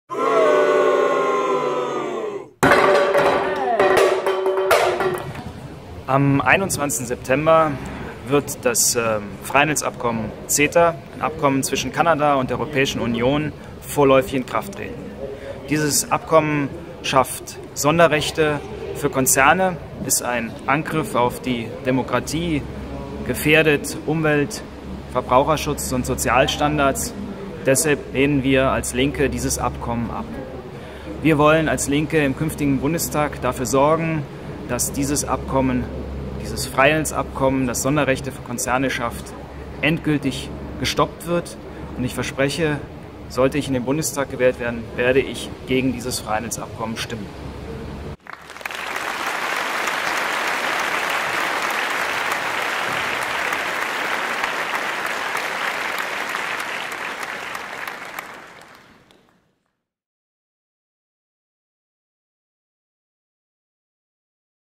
8. Sep2017: Pascal Meiser zum Freihandelsabkommen CETA
Am 21. September 2017 soll CETA, das Freihandels- und Investitionsschutzabkommen zwischen der Europäischen Union und Kanada "vorläufig" in Kraft treten. Die Zustimmung des Bundestags und des Bundesrats soll dann erst später eingeholt werden.

In unserem Video sagt Pascal Meiser, warum wir als DIE LINKE dieses Abkommen weiterhin stoppen wollen: es schafft Sonderrechte für Konzerne, höhlt die Demokratie aus und bedroht, Umweltschutz-, Verbraucherschutz- und Sozialstandards. Deshalb beteiligen wir uns als LINKE am bundesweiten Aktionstag gegen CETA am 16. September und wir werben auch deshalb um Eure Unterstützung bei der Bundestagswahl am 24. September.